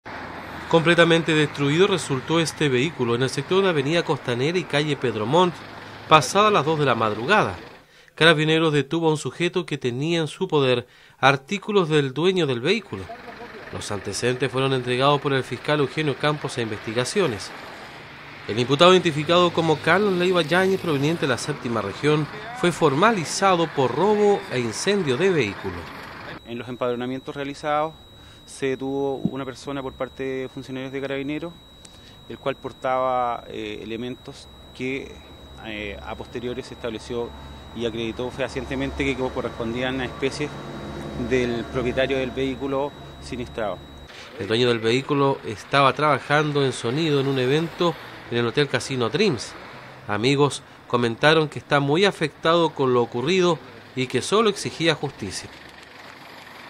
PRESO POR ROBAR Y QUEMAR UN AUTOMOVIL EN LA COSTANERA
El imputado Carlos Leiva, fue enviado a prisión por haber cometido este delito en Costanera a la altura de calle Pedro Montt.